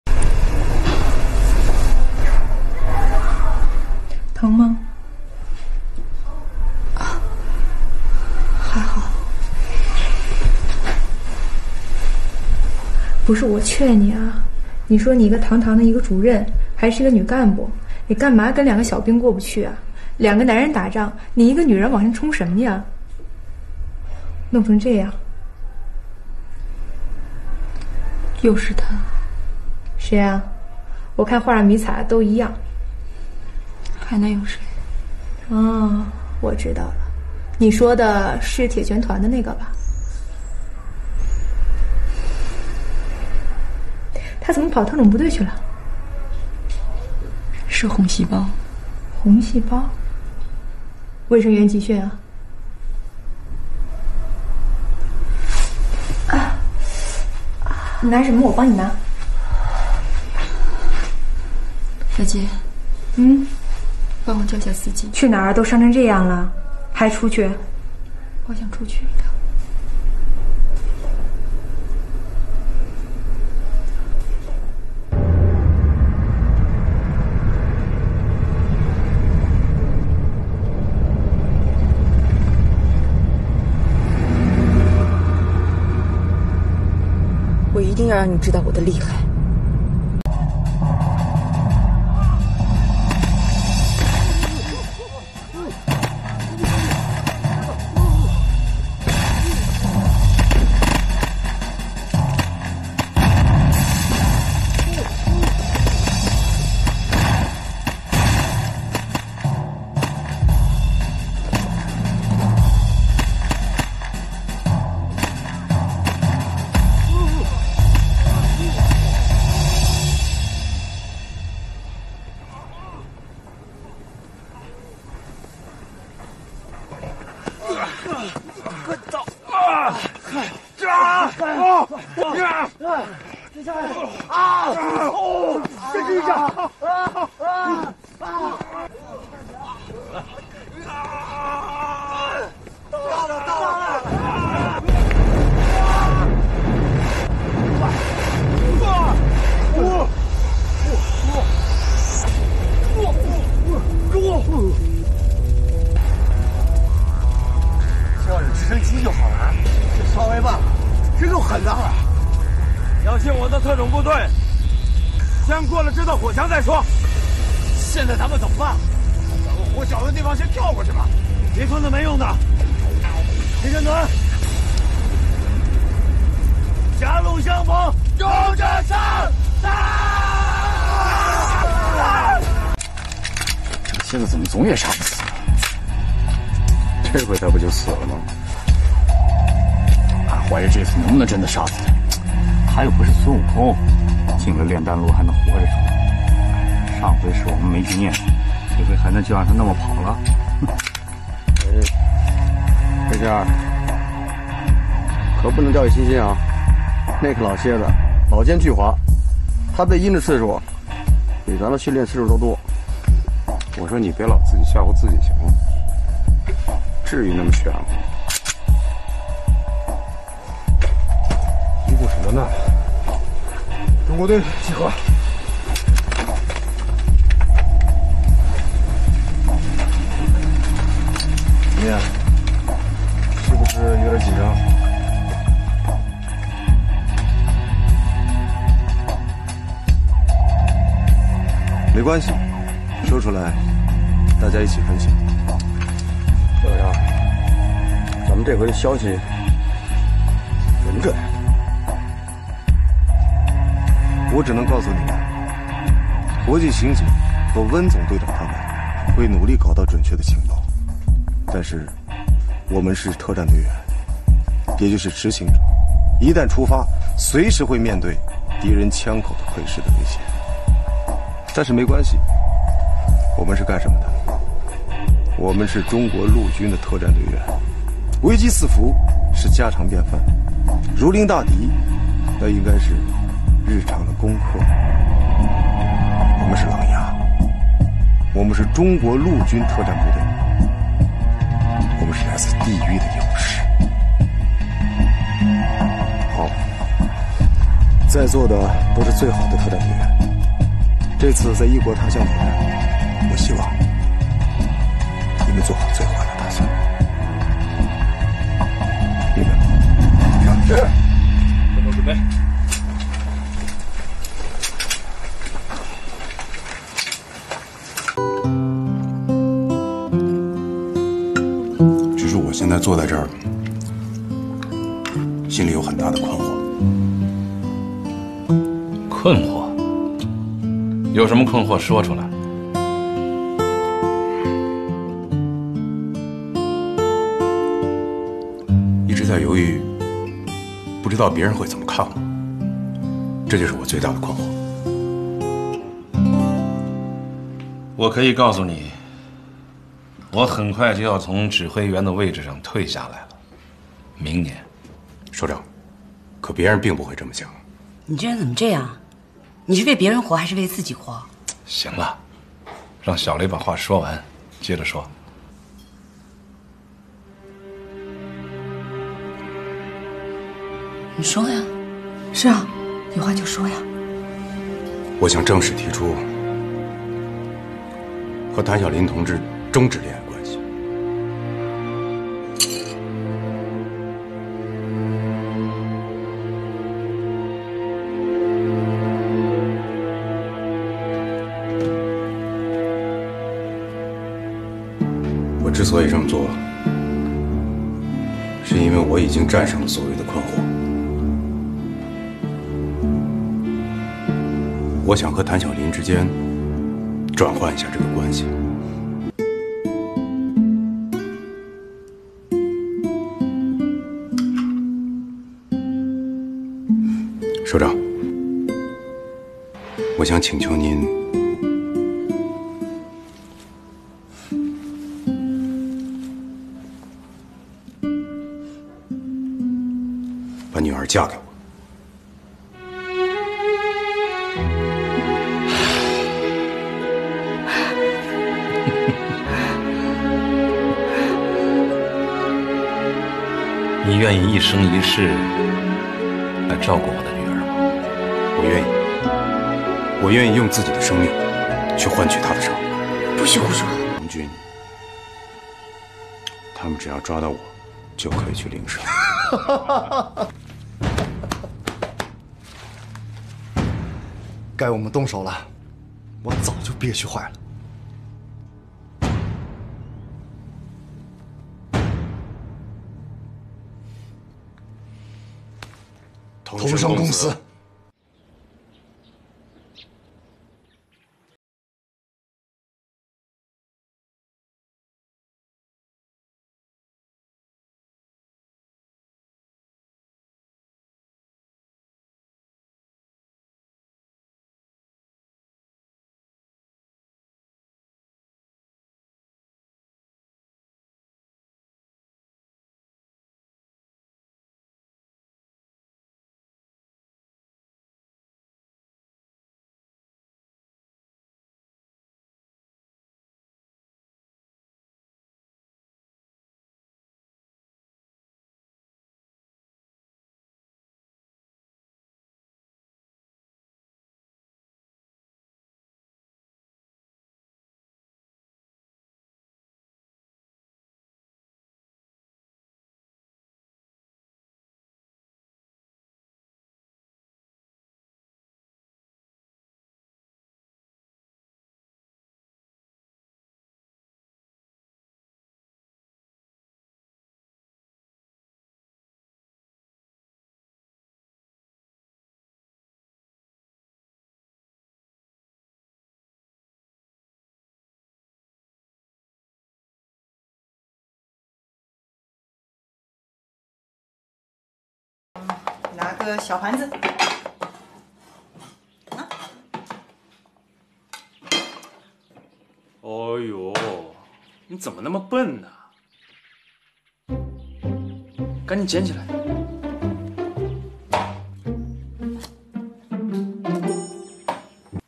女首長被新兵一頓爆揍，結果內心卻很喜悅，越打越愛
👉所有内容皆为正版授权内容
感謝您的訂閱支持：
精彩好劇，讓我們一起吐槽點贊看個爽！